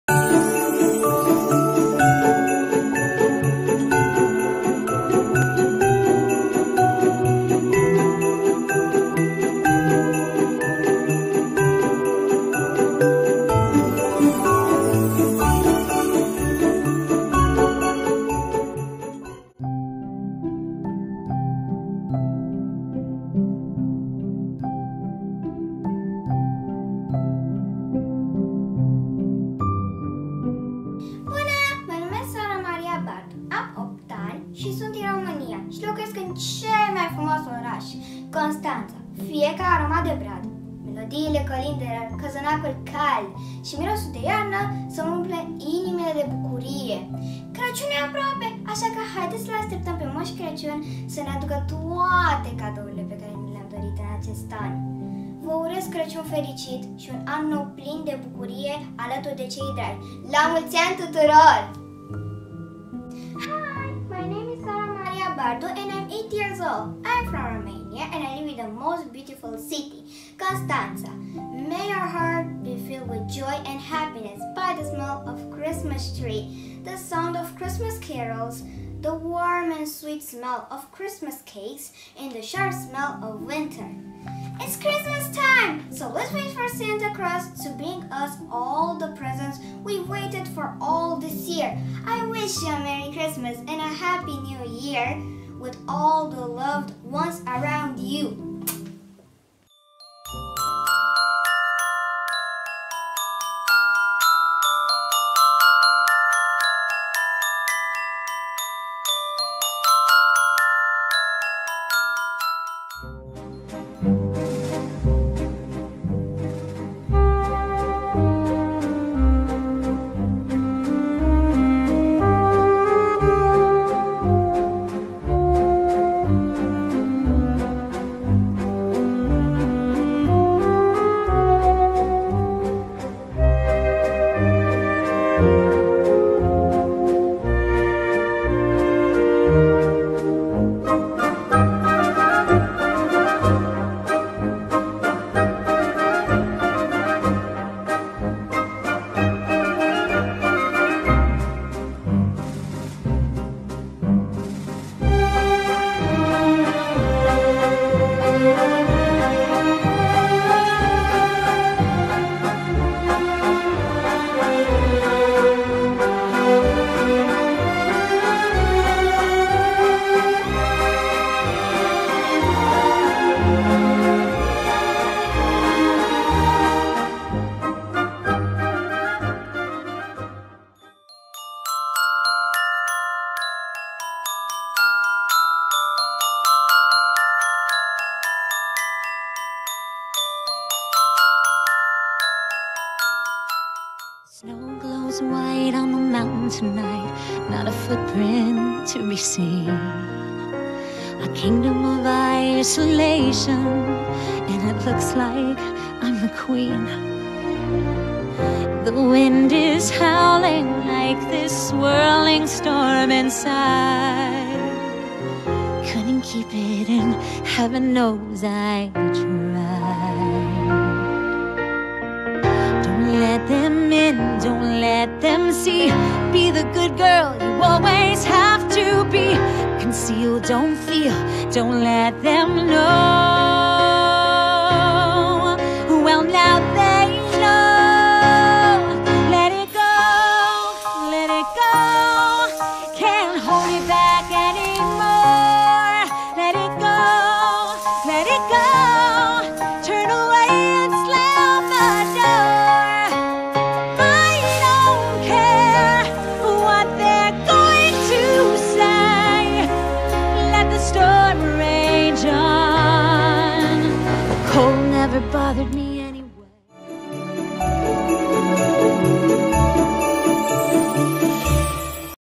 Sara Bardu, ROMANIA. Winter Ball with TV START. 2020, December - 2021, January
Meet the participant of the Online Beauty Contest "Prince and Princess of the Ball"
Sara Bardu, ROMANIA - 8 years old
1. Presentation «Video congratulations on New Year’s and Christmas holidays».
2. Defile “Ballroom costume".
3. Defile " The Fabulous Character "
International online project. «Winter Ball with TV START »
TV START International TV Festival's Movement
TV START- FESTIVAL
Organizers:
Liliya Sheкhanina
Aleksandr Litvinenko